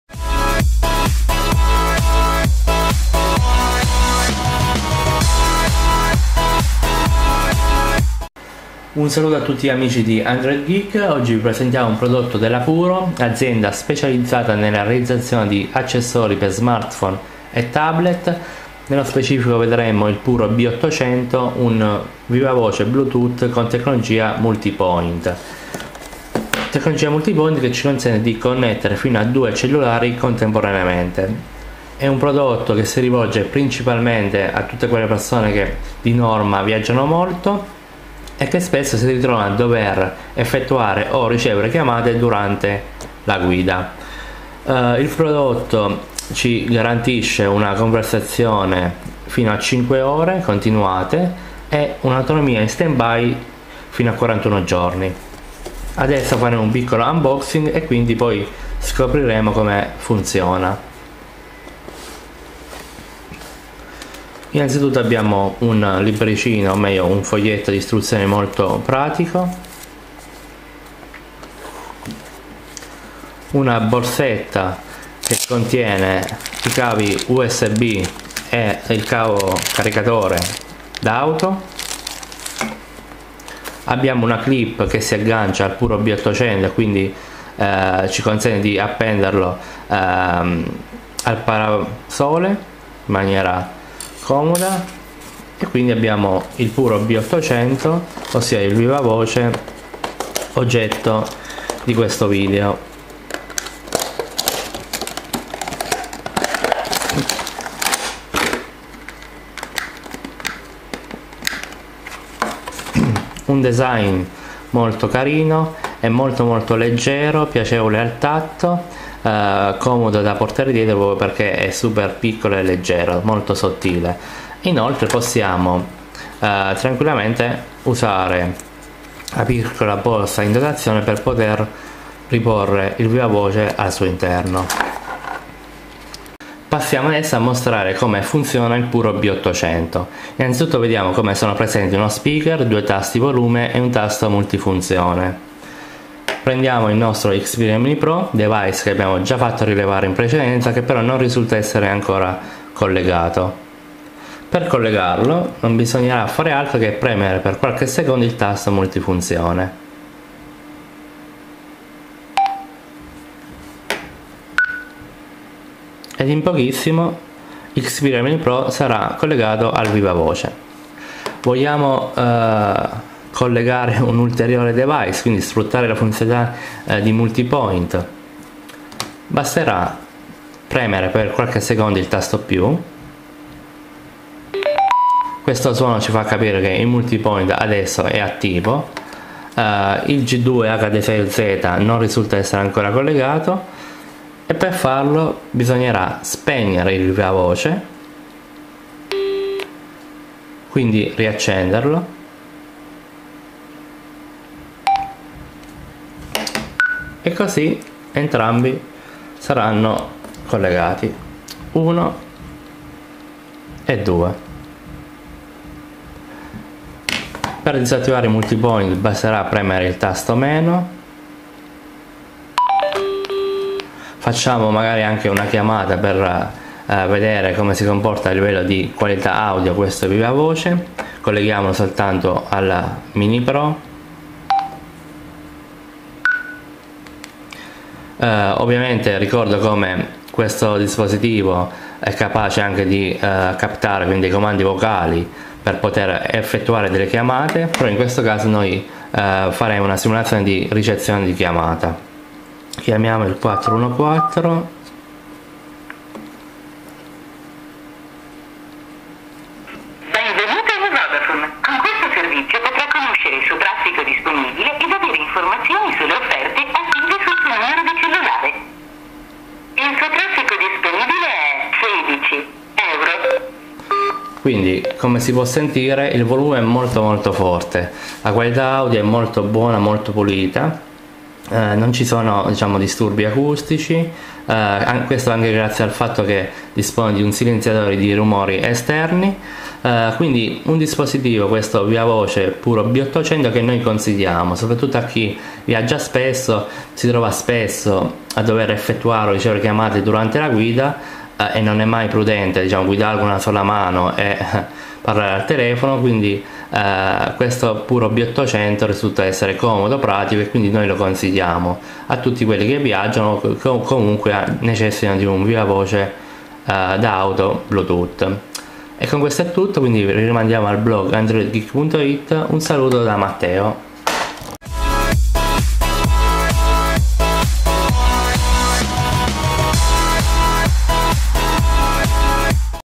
Recensione PURO B800 - Vivavoce Bluetooth Multipoint
Recensione del PURO B800, un avanzato Vivavoce Bluetooth con tecnologia Multipoint caldamente consigliato a tutti quelli che hanno necessità di effettuare e ricevere chiamate durante la guida.